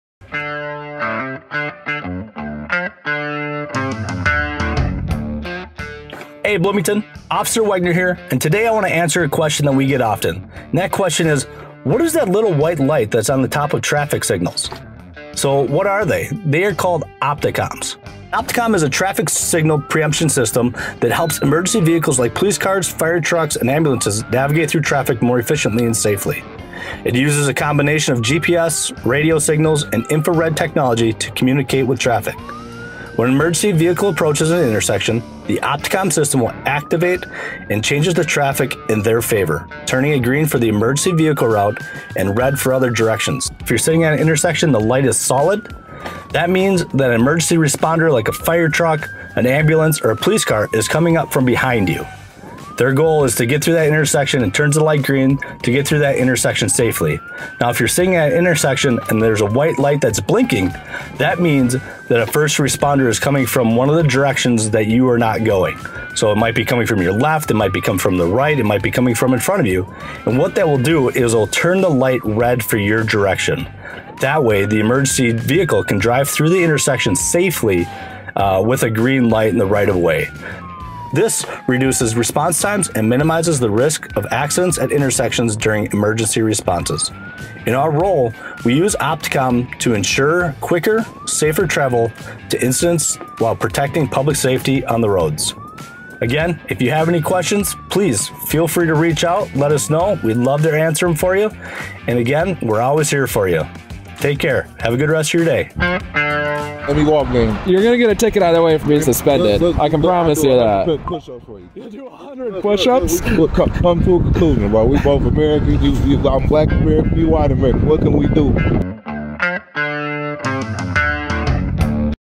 So that’s how cops control traffic lights!
How Do Police Control Traffic Lights? A
Ever noticed emergency vehicles changing red lights
to green? That's Opticom in action! This system uses a strobe or infrared signal to communicate with traffic lights, giving police, fire, and EMS the right-of-way. It helps emergency responders reach their destination faster and safer. Watch to see it in action!